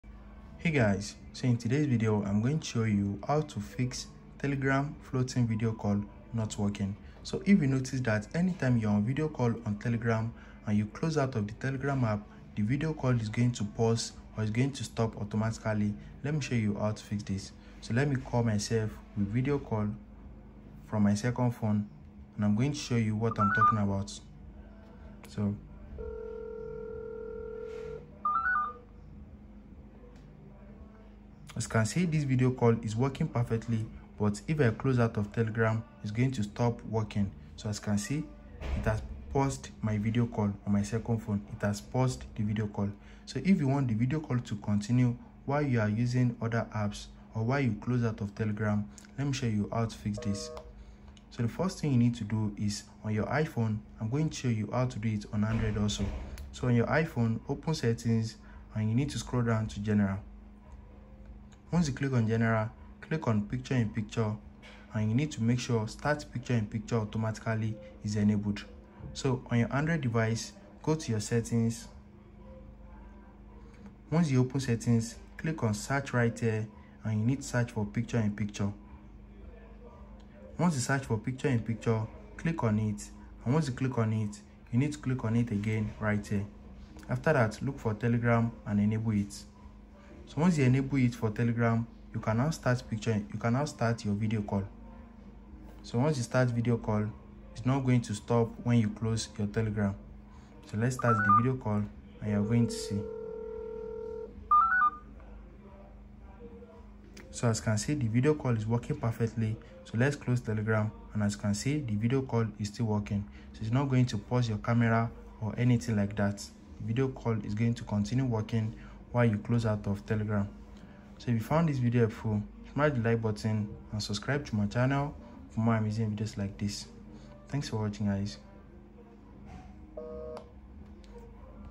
How to Fix Telegram Floating Video Call Not Working (iPhone & Android)
Encountering problems with Telegram's floating video call feature on your iPhone or Android device? Learn how to resolve it with our step-by-step guide! Discover troubleshooting tips to ensure seamless video calling experience.